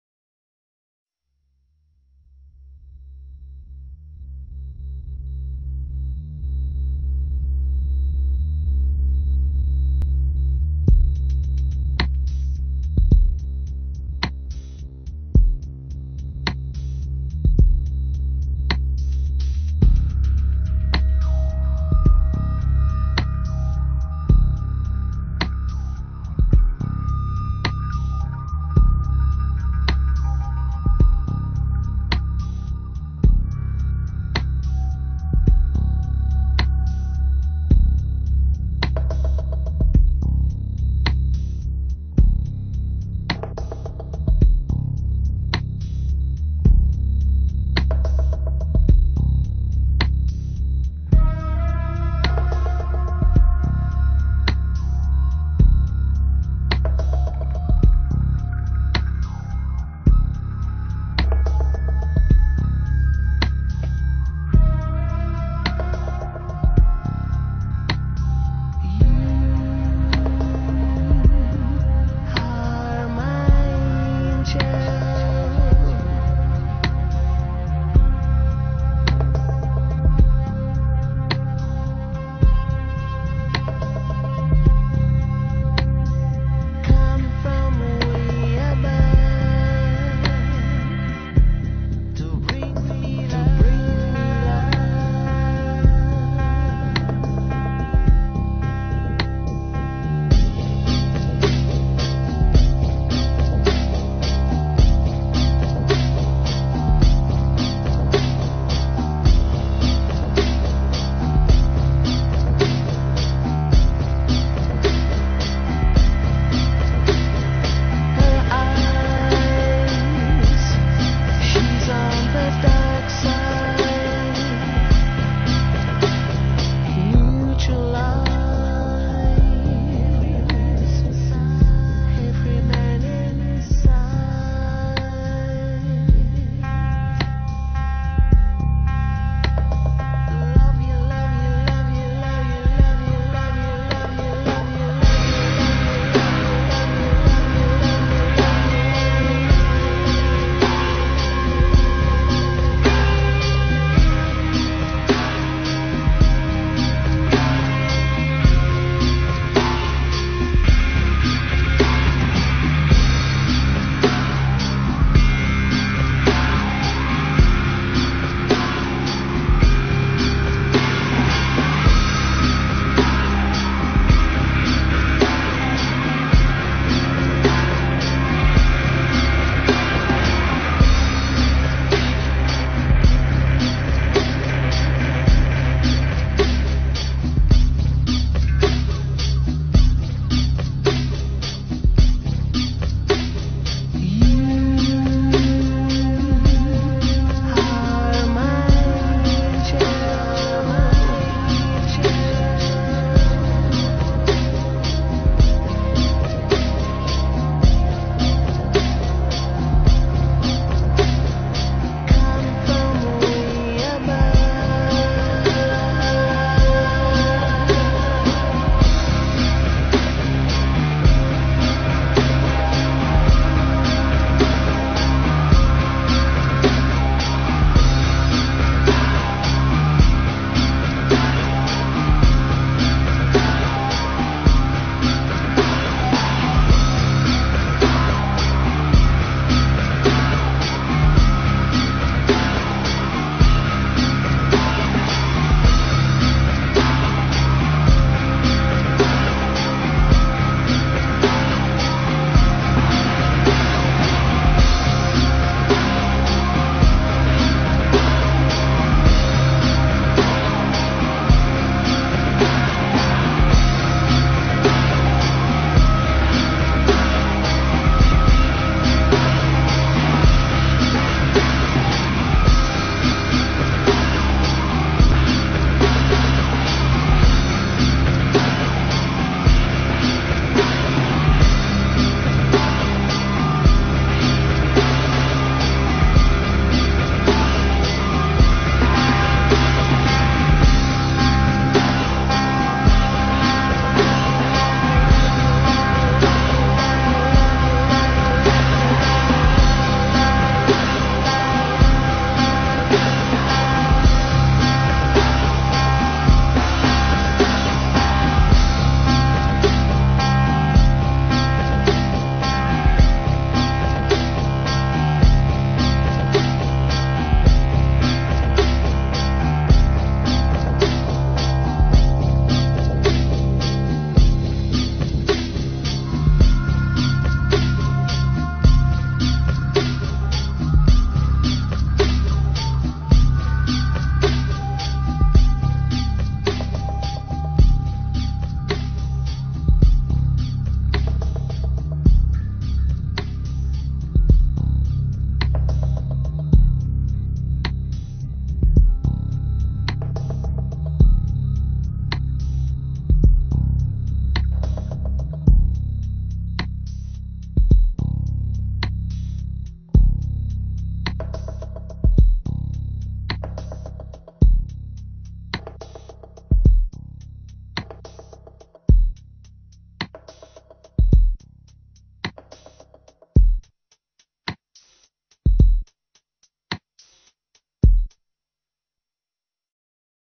Angels - Fathers4Justice London Demo. Met Police Helicopter footage
A clip of various demos across London featuring Met Police Helicopter footage used as evidence. Up the Dads! Give us our equality!! :O)